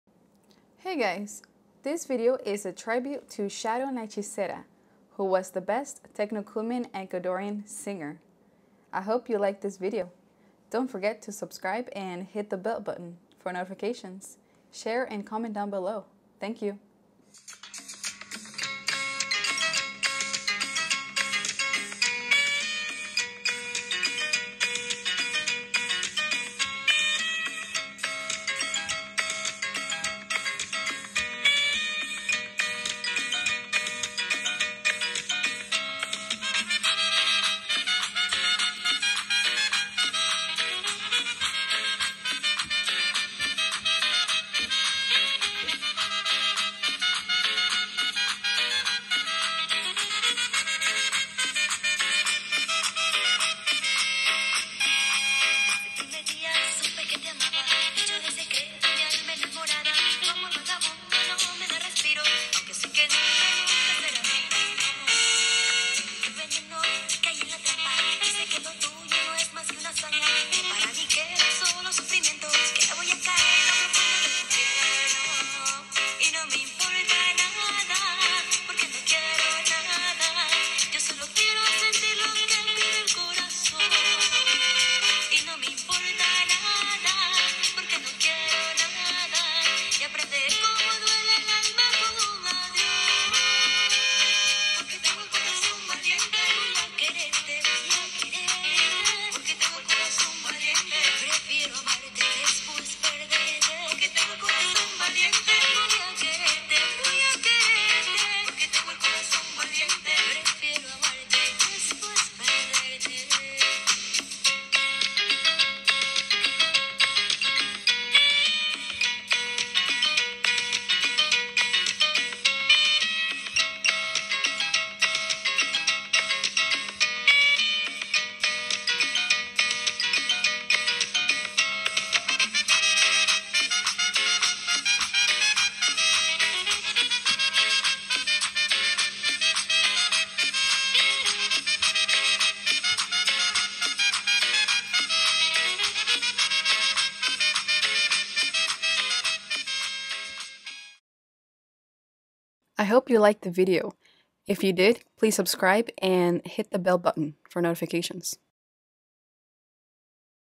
Sharon La Hechicera Tribute
Hey guys, this video is a tribute for Sharon La Hechicera who was the best Techno Cumbia Ecuadorian singer. I hope you like this video.